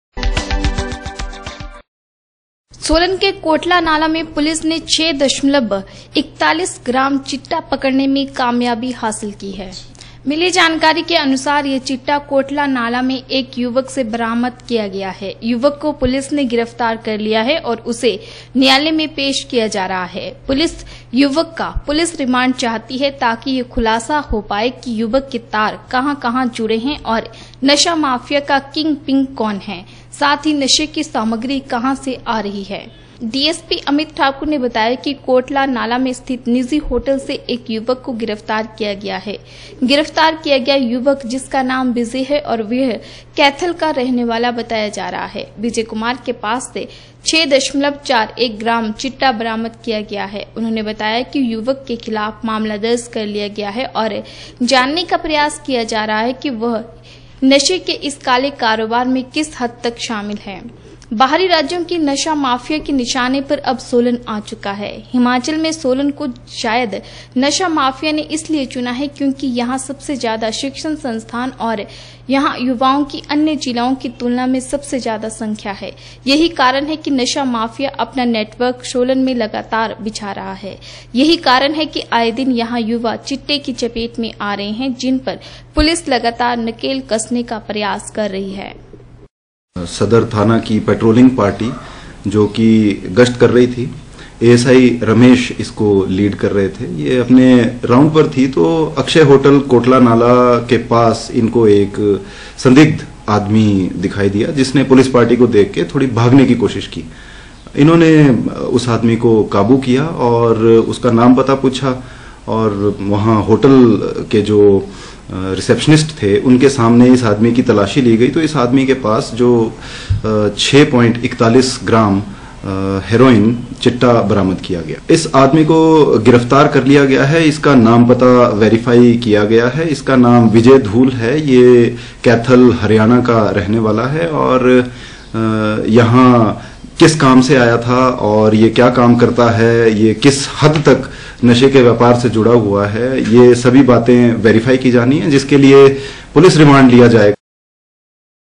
सोलन के कोटला नाला में पुलिस ने 6.41 ग्राम चिट्टा पकड़ने में कामयाबी हासिल की
सोलन के कोटला नाला में पुलिस ने 6.41  ग्राम    चिट्टा पकड़ने में कामयाबी हासिल की है  मिली जानकारी के अनुसार यह चिट्टा कोटला नाला में एक युवक से बरामद किया है द्य युवक को पुलिस ने गिरफ्तार कर लिया है और उसे आज न्यायालय में पेश किया जा रहा है  द्य पुलिस युवक का पुलिस रिमांड चाहती है ताकि यह खुलासा हो पाए कि युवक के तार कहाँ कहाँ जुड़े है और नशा माफिया का किंग पिन कौन है साथ ही नशे की सामग्री कहाँ से आ रही है